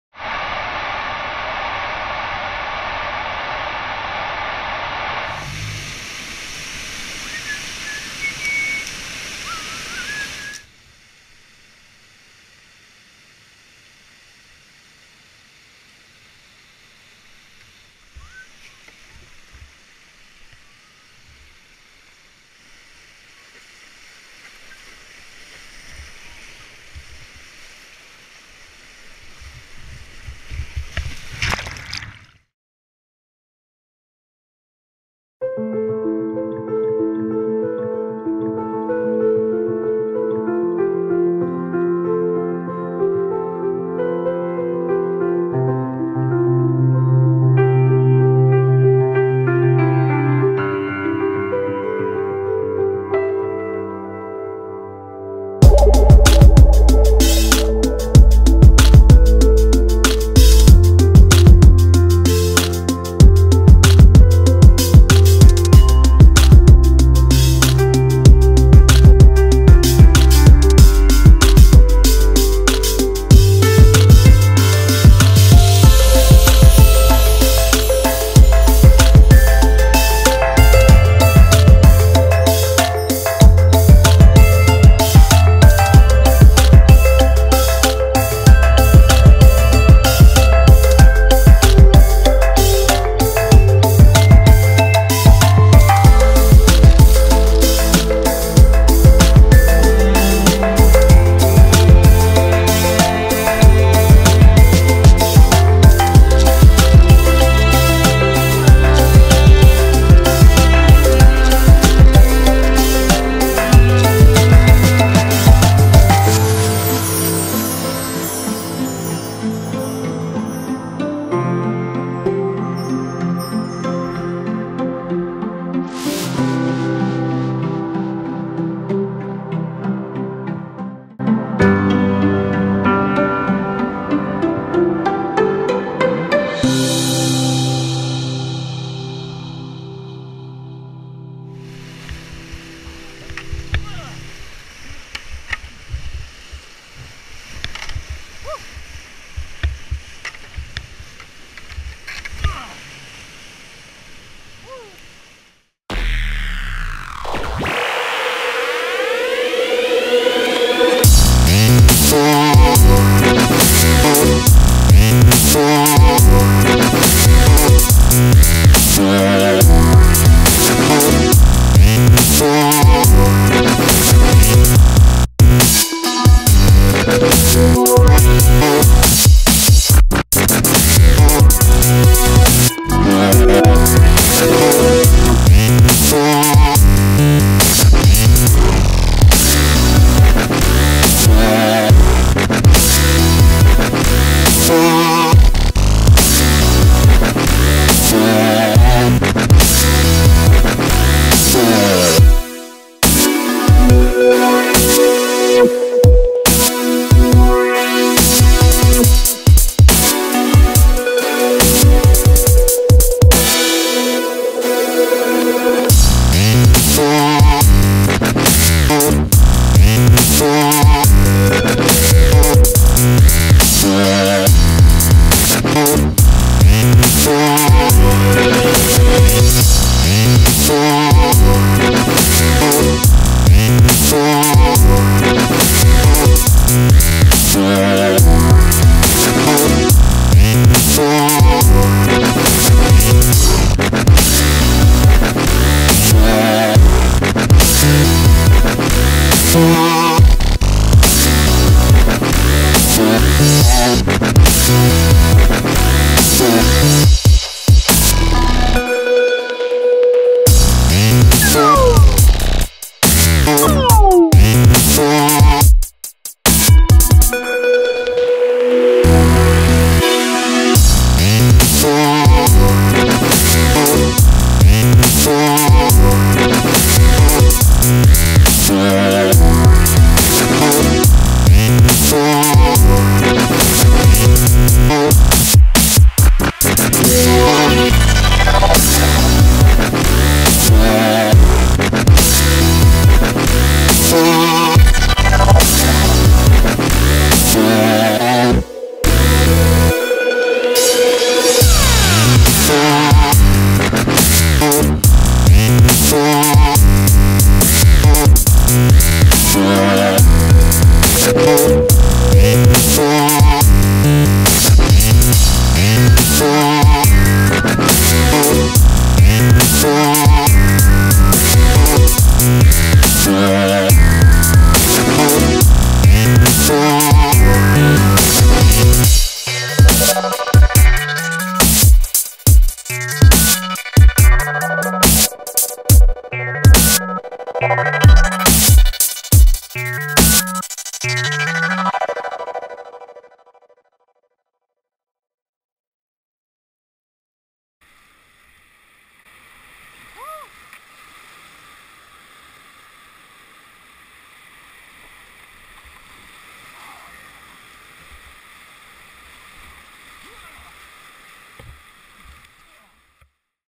Packrafting Peter's Creek
A low water trip down Peter's Creek in Chugiak, Alaska